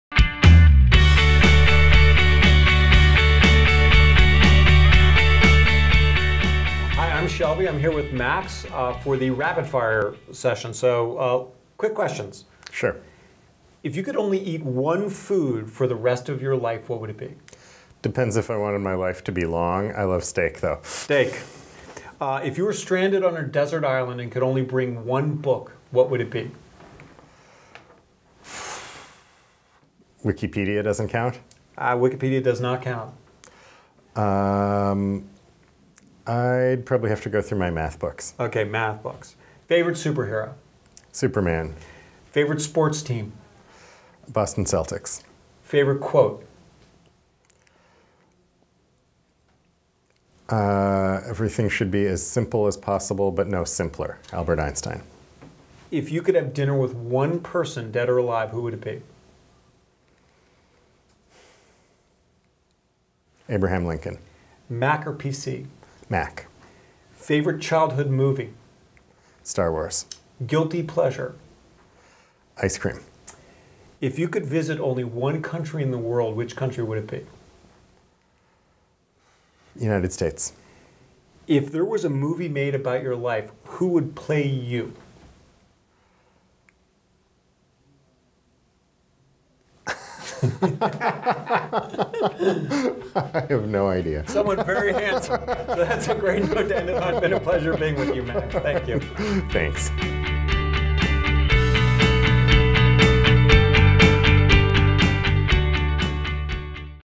Max Schireson, 10gen: Rapid Fire Questions
MAX SCHIRESON, PRESIDENT at 10GEN
Rapid Fire Questions

Schireson (@mschireson) is the President of 10gen, a company which offers production support, training, and consulting for open source databases. Prior to joining 10gen, Max spent 7 years at MarkLogic, provider of a database for unstructured information, where he played a variety of executive roles including Chief Operating Officer. Under Schireson's leadership, MarkLogic ranked as the fourth fastest growing software company in Silicon Valley in Deloitte's 2008 Technology Fast 50. Prior to MarkLogic, Max spent 9 years at Oracle where he served as Chief Applications Architect and Vice President, ECommerce and Self Service Applications. He is a national co-chair of Tech4Obama.